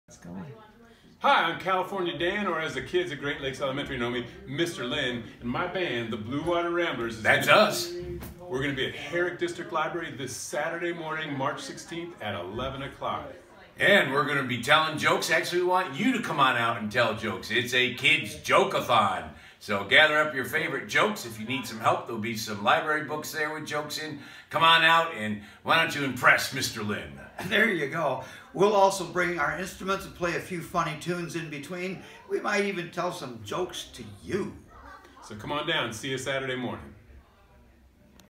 Blue Water Ramblers- Kids Joke Time Herrick District Library
Please share this 38-second video with your class. It would be awesome to pack the house with GLakes kids.

Kids are invited to take the stage during LaughFest’s official event at Herrick District Library, Kids Joke Time Saturday, March 16, 11:00 - 12:00. Young comedians are encouraged to gather their favorite jokes and take the mic. What ensues makes this one of the most entertaining events during the festival!

Please arrive 30 minutes early for registration if your little jokester plans to take the stage.

Supported with Kids Music and fun by the Blue Water Ramblers!